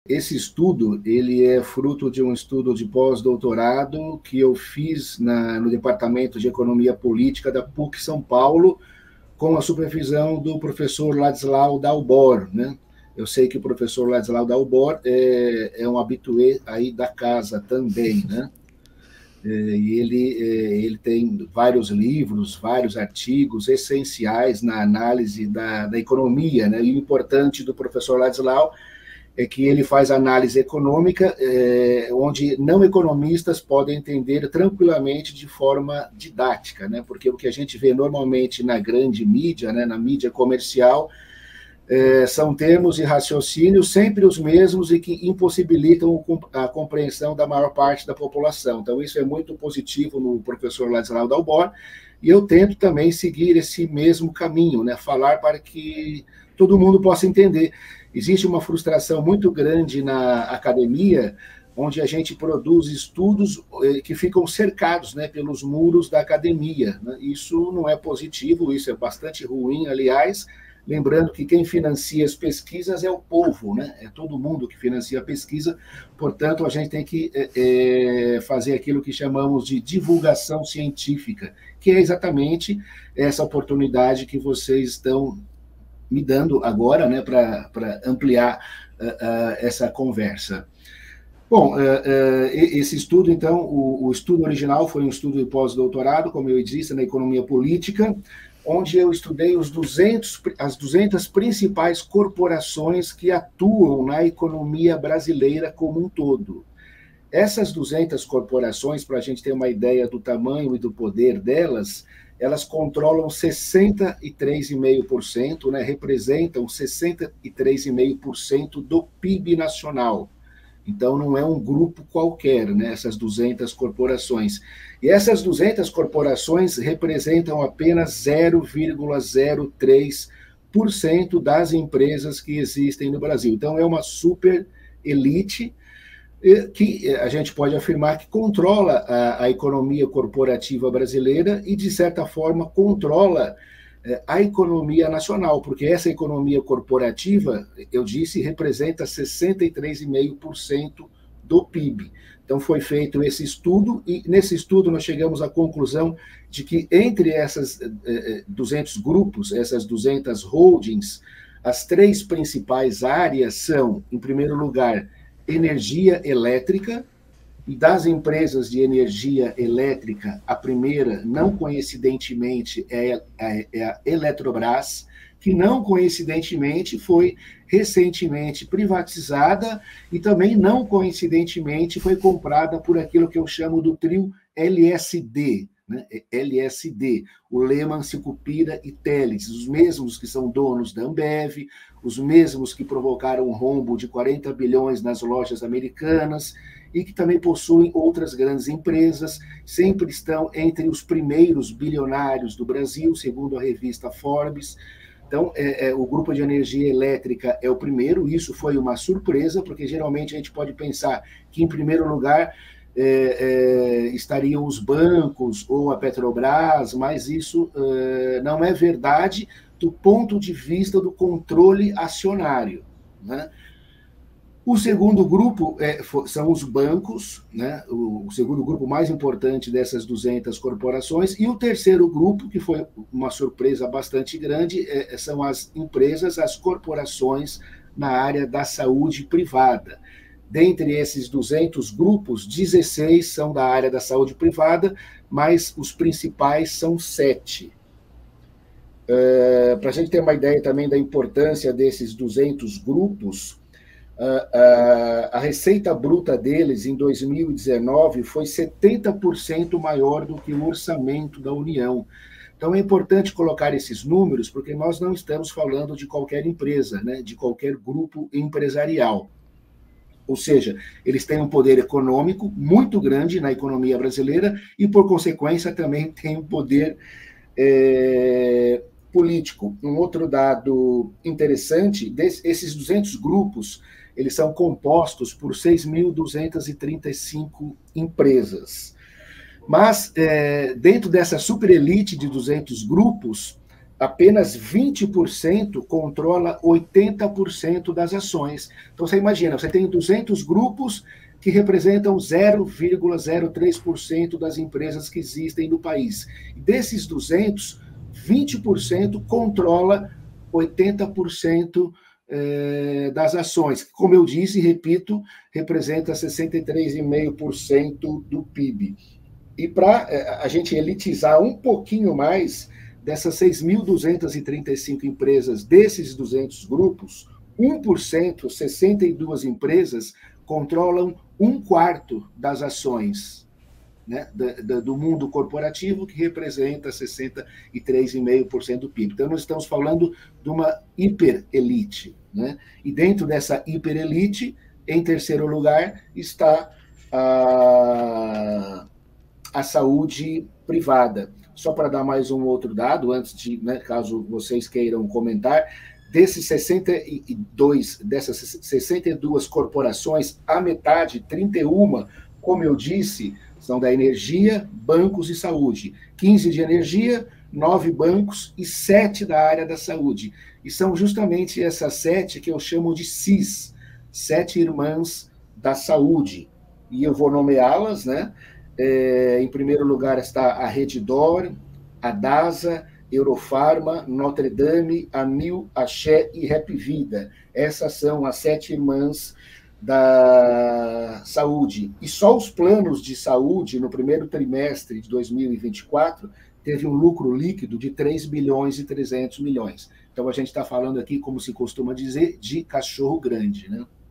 A saúde privada brasileira nas mãos de poucos
No Brasil, a saúde privada é dominada atualmente por um oligopólio de sete holdings, compondo uma reduzidíssima e poderosa classe na liderança e comando decisivo da economia brasileira. O pesquisador no Centro de Estudos de Saúde Coletiva (Cesco) e pós-doutorando em Saúde Pública pela USP Eduardo Magalhães Rodrigues realizou um estudo que desvela os diversos nós que ligam esses gigantes econômicos.

OUTRAS PALAVRAS